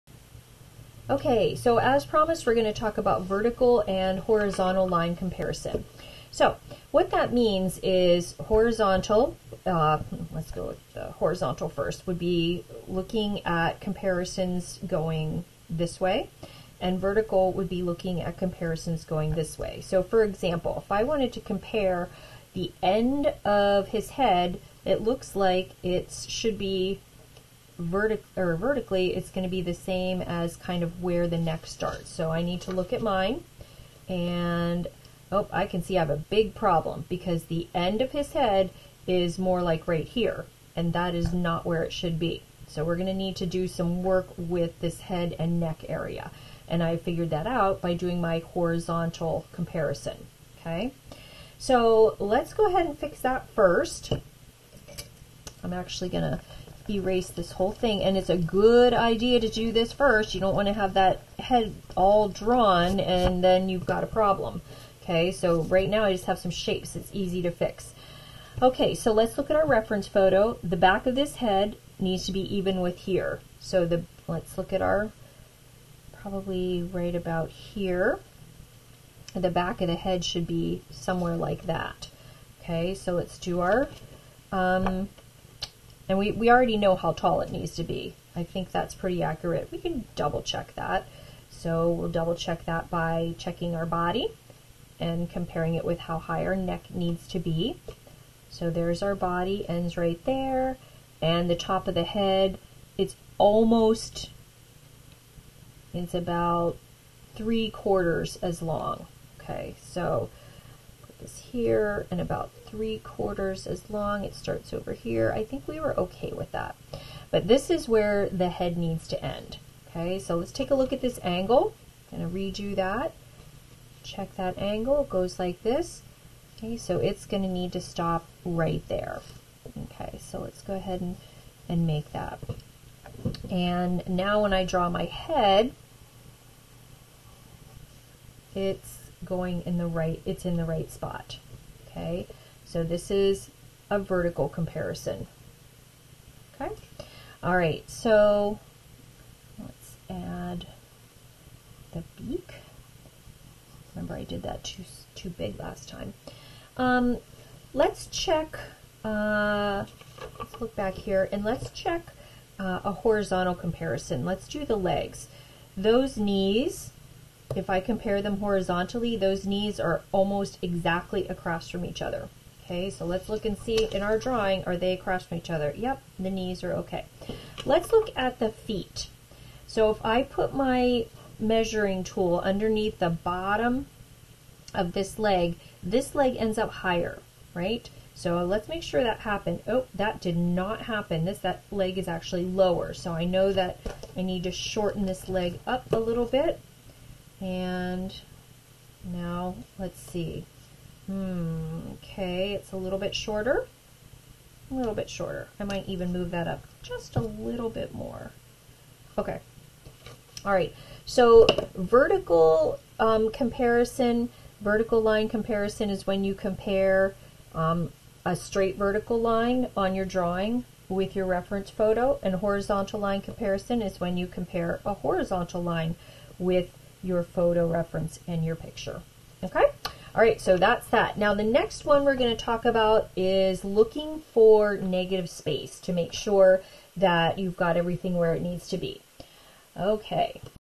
vertical and horizontal line comparison
This video talks about how to use vertical and horizontal lines to compare parts of your drawing.